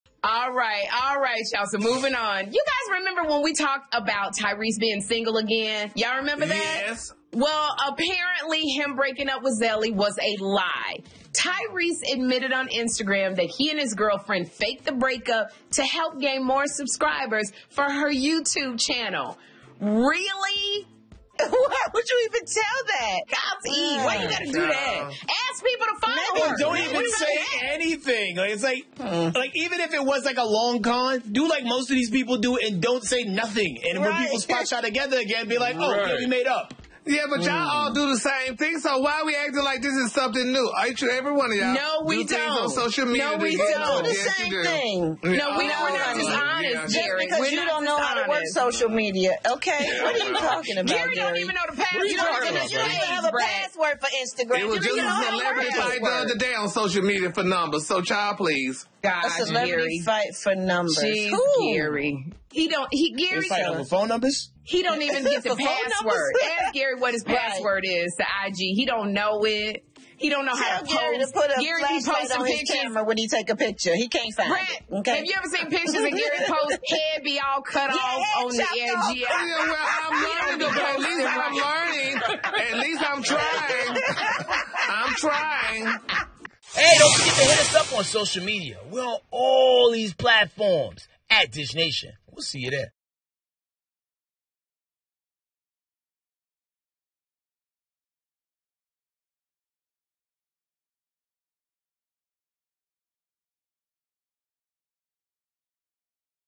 Tyrese and Girlfriend Admit They Faked Breakup to Get More Followers for Her YouTube Channel
Oh, baby boy... Tyrese is back with his boo. "Cryrese" admits he faked his breakup with ZelieTimothy for more followers on her YouTube channel. Tune in to see why GaryWithDaTea thinks all celebrities are guilty of doing something like this!

HOSTS
ATLANTA: